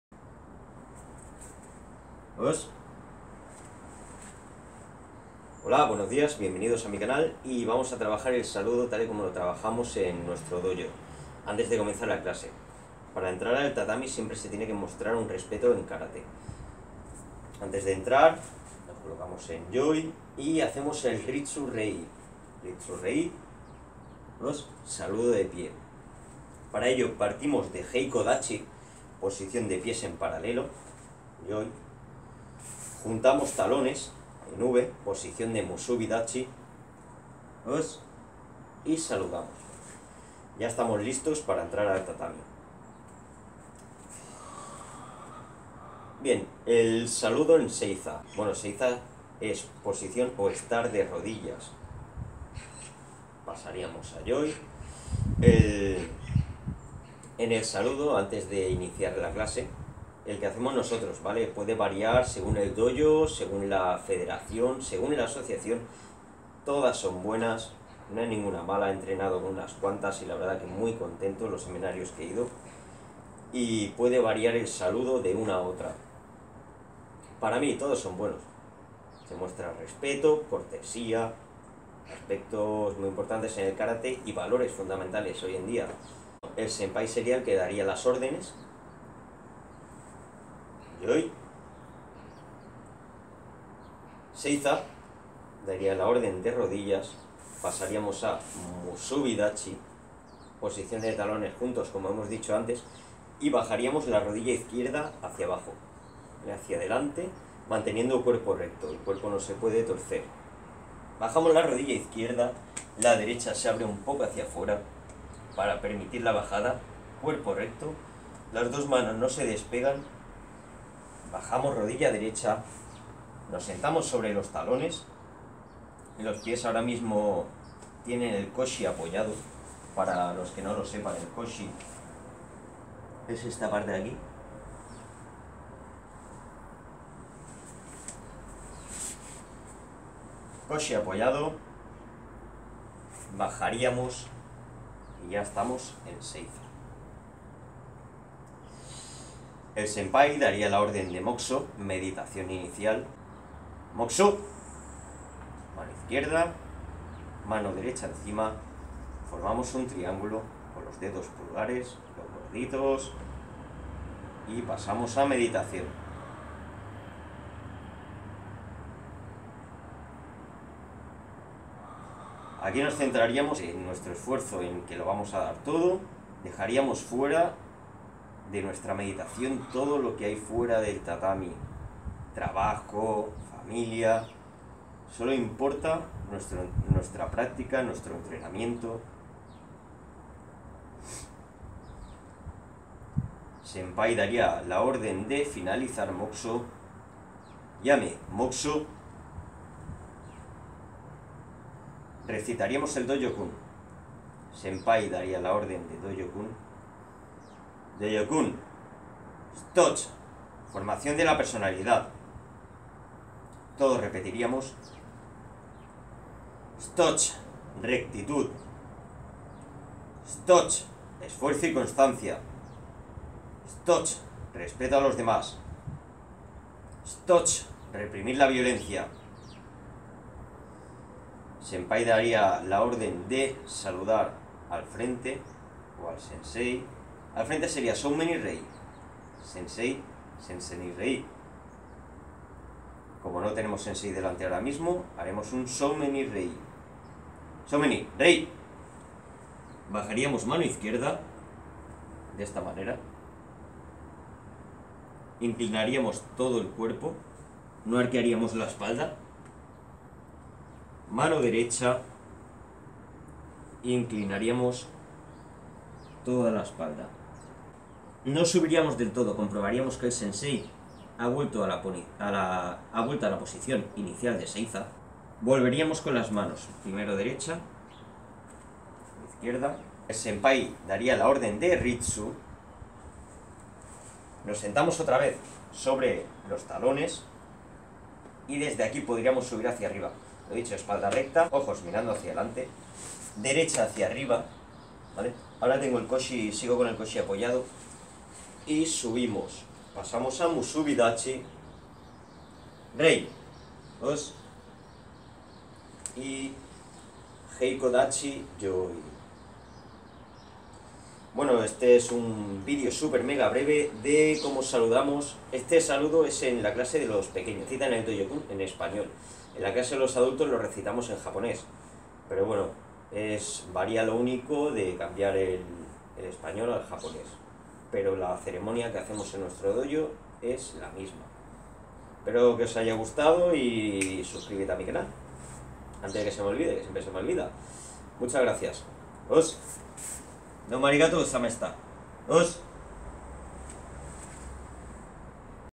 SALUDO EN KARATE y DOJO KUN
Un breve vídeo de cómo saludamos en nuestro Dojo.

Oss